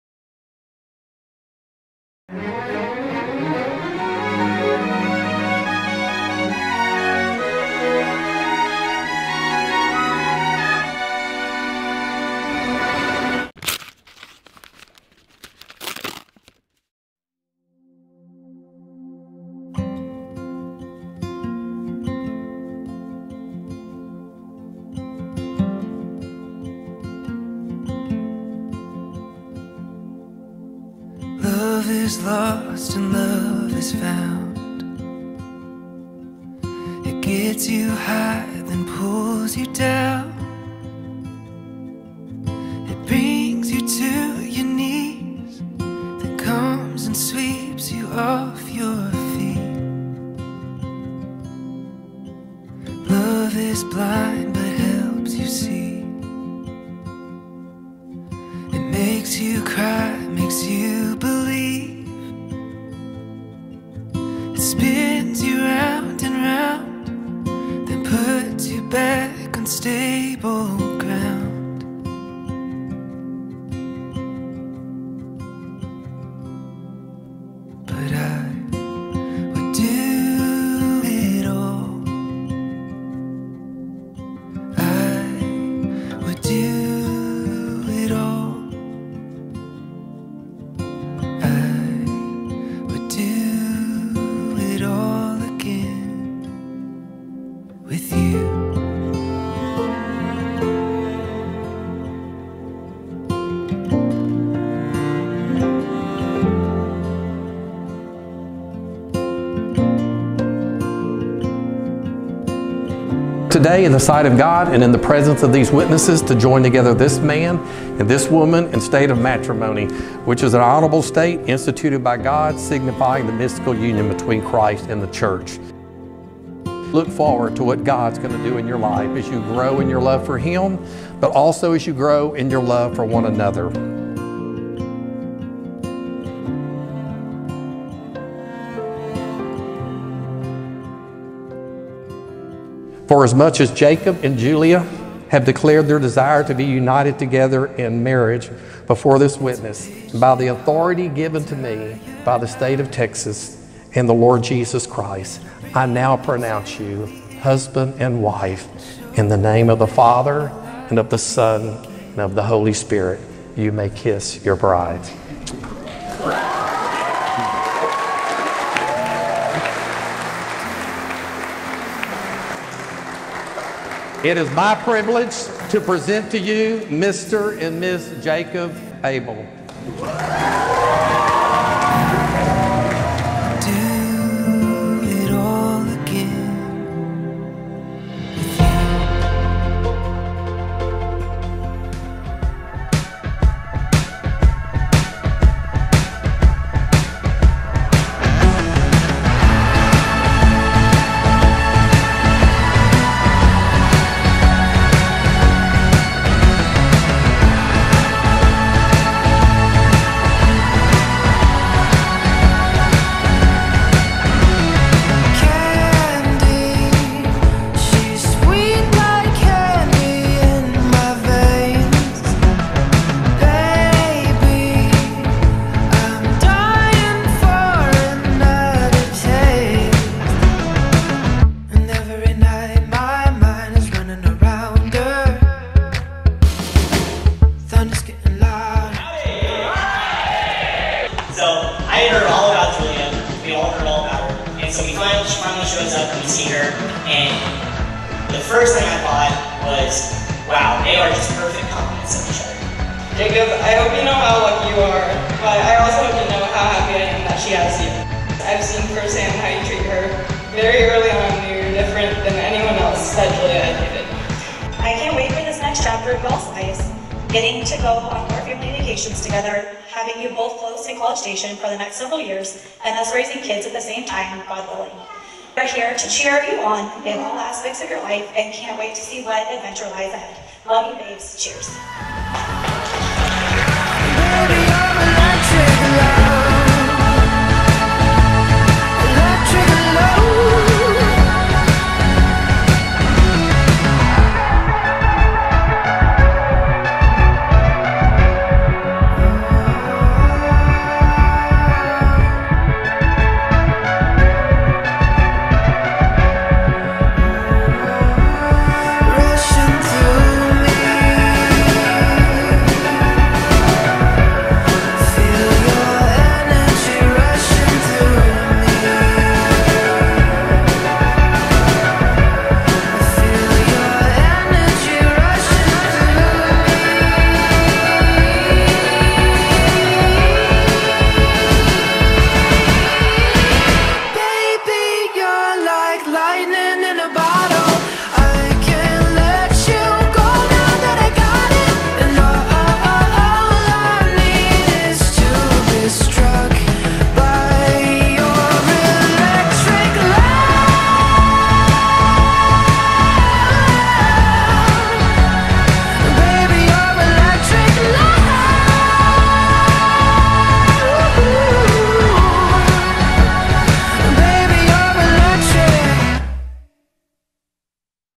The Milestone - Aubrey, TX - Julia & Jacob's Wedding
Julia + Jacob - Sneak Peek 🤍

Congratulation to Mr. & Mrs. Abell. On 7/7/22 these two tied the knot at The Milestone in Aubrey, Texas. This day was absolutely breathtaking! More to come next week of their ceremony + reception! 🤍

Venue: The Milestone - Aubrey Texas

All images and videos are property of Rare Gem. Please do not crop, edit, or share images without crediting @ Rare Gem Photo. Thank you!